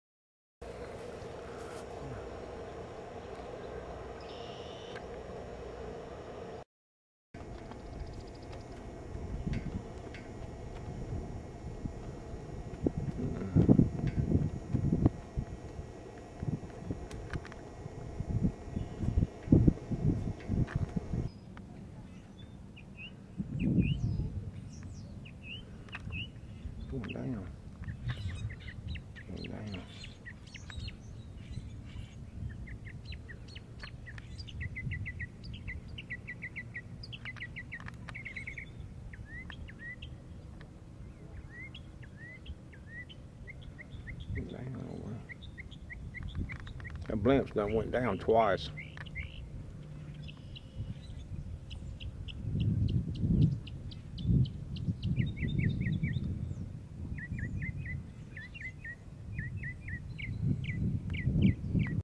Direct TV Blimp Flying Low on J. Percy Priest Lake near Fate Sanders Marina
Smyrna Airport